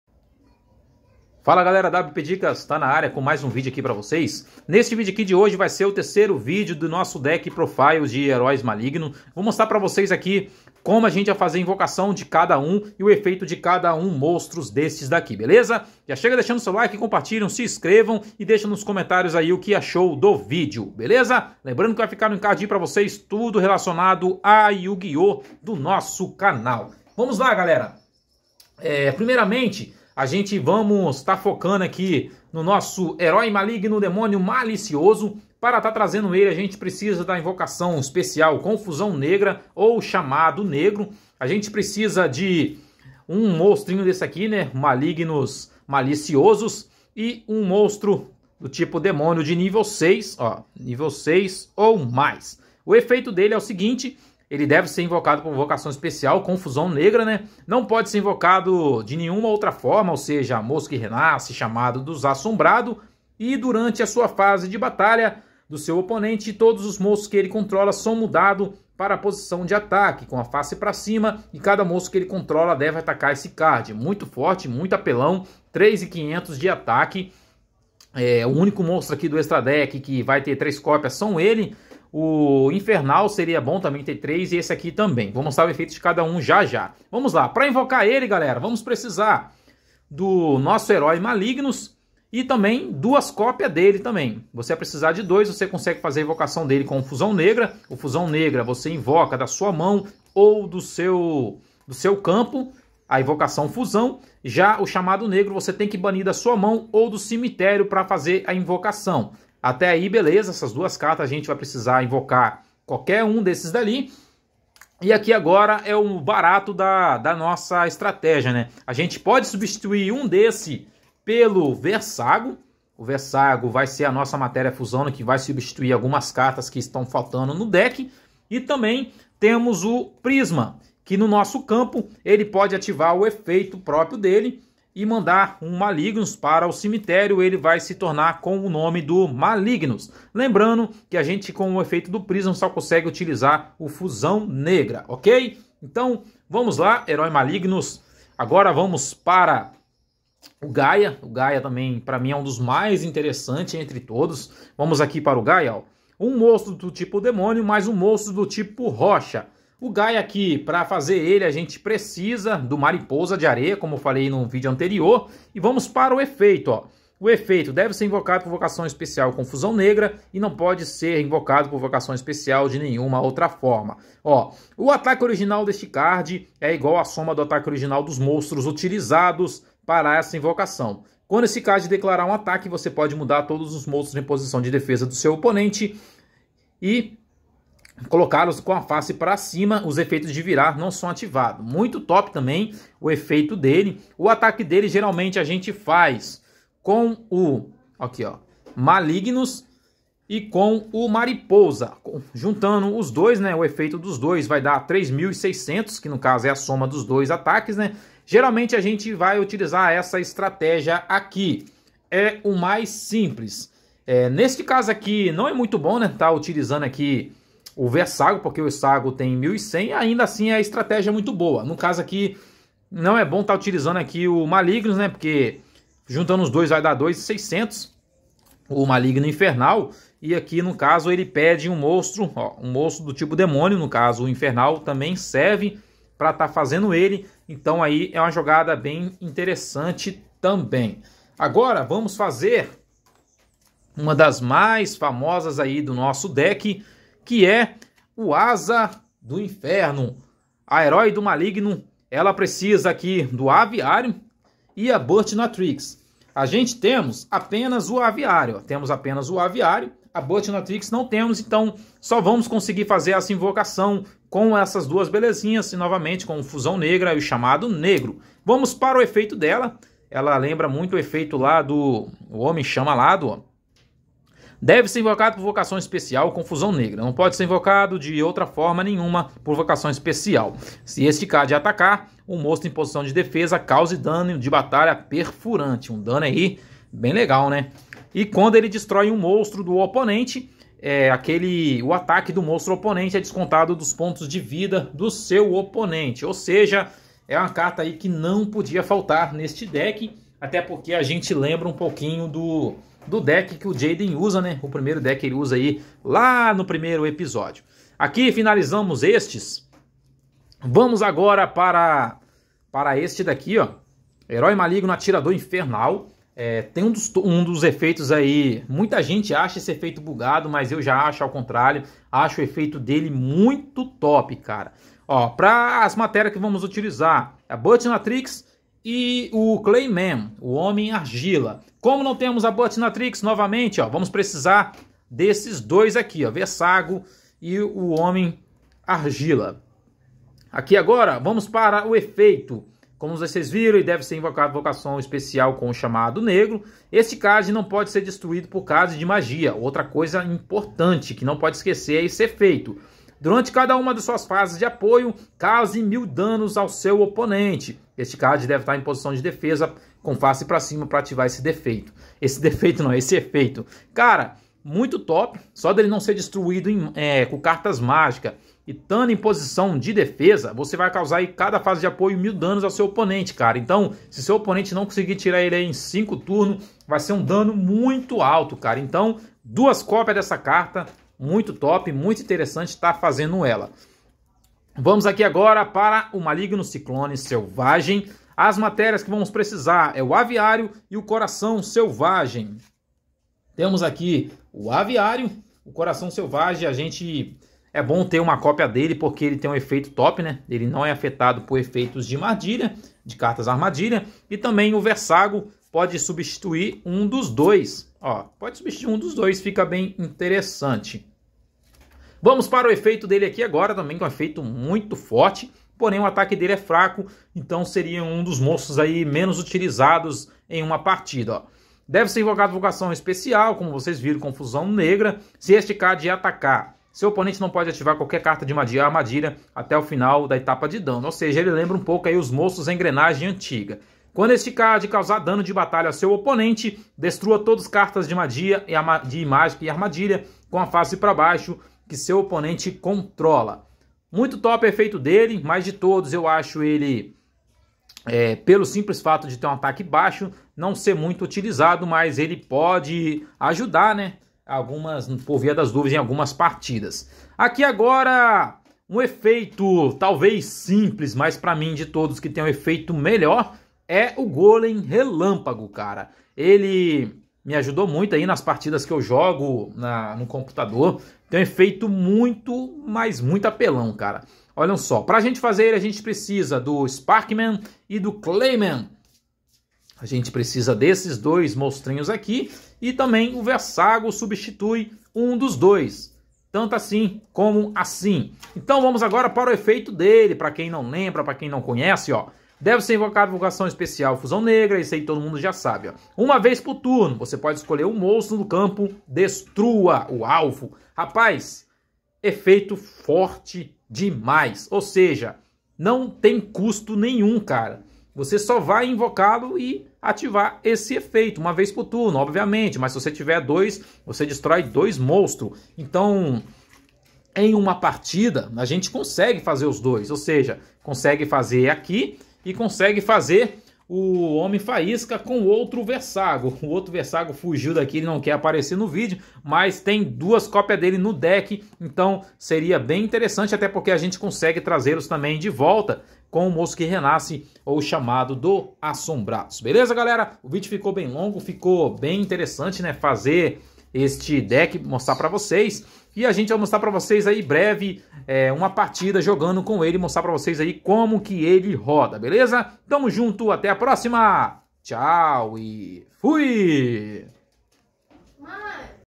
YUGIOH - EXTRA DECK - HEROI MALIGNO
FALA GALERA WP DICAS NA AREA
NESSE VIDEO MOSTRO A VOCES COMO FUNCIONA UM POUCO DO NOSSO  EXTRA DECK.
LEMBRANDO QUE JOGO NA DIVERSÃO E NAO É MUITO COMPETITIVO POREM DA PRA GANHAR DE BOA.
NO PROXIMO VIDEO VOU MOSTRAR MAIS UM NOVO DECK.